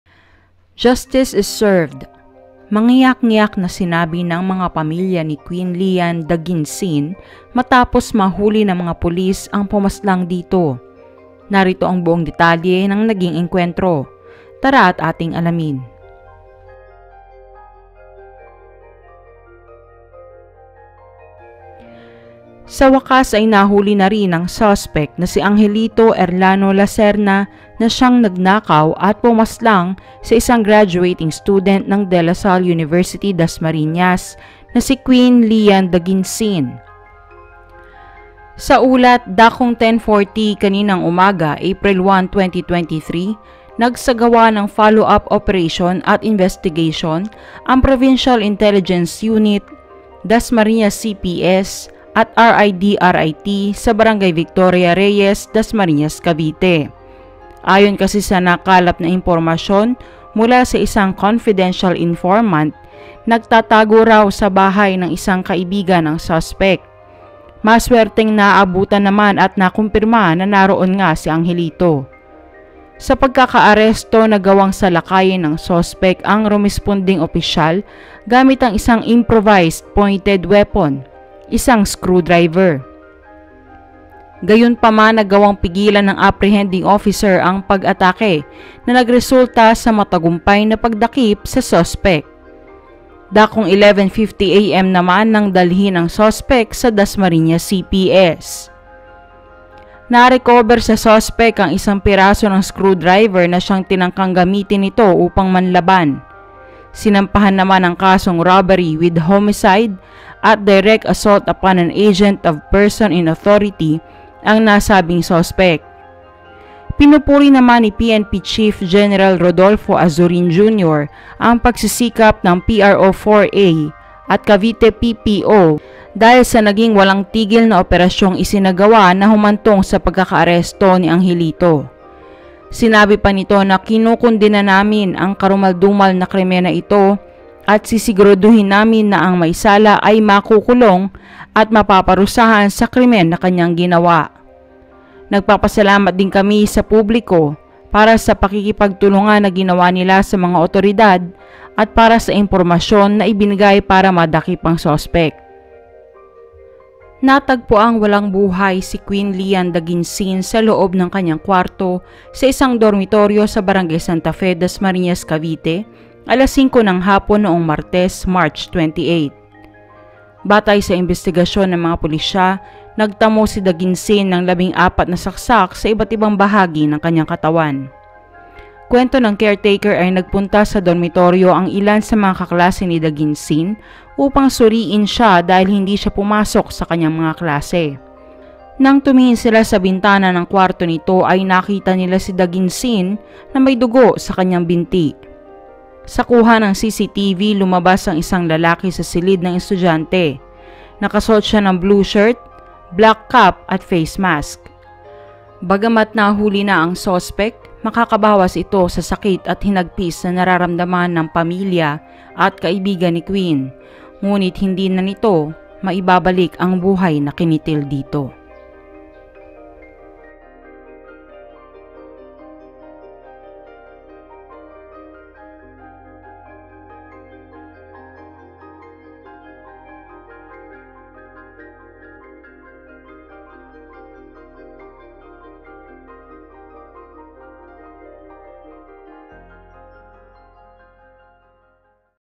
BUONG DETALYE! SA PAGTIMBOG SA SUSPEK NG KASO QUEEN LEANNE DAGUINSIN
Sa wakas ay nahuli na rin ang suspek na si Angelito Erlano Lacerna sa ginawa nito sa estudyante ng De La Salle University Dasmarinas na si Queen Leanne Daguinsin